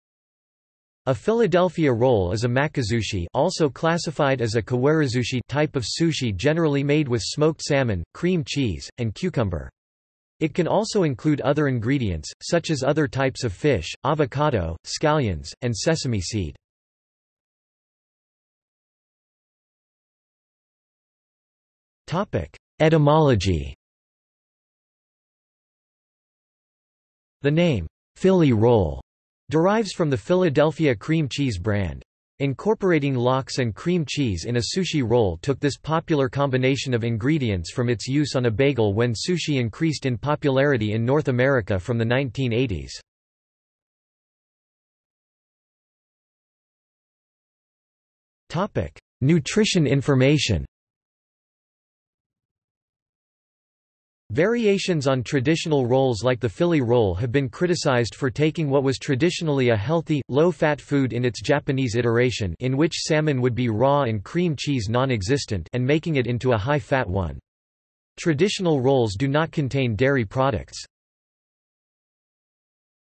Cheese maki | Wikipedia audio article
This is an audio version of the Wikipedia Article:
Cheese maki

00:00:18 undefined
00:00:44 undefined

- Socrates

SUMMARY
A Philadelphia roll is a makizushi (also classified as a kawarizushi) type of sushi generally made with smoked salmon, cream cheese, and cucumber. It can also include other ingredients, such as other types of fish, avocado, scallions, and sesame seed.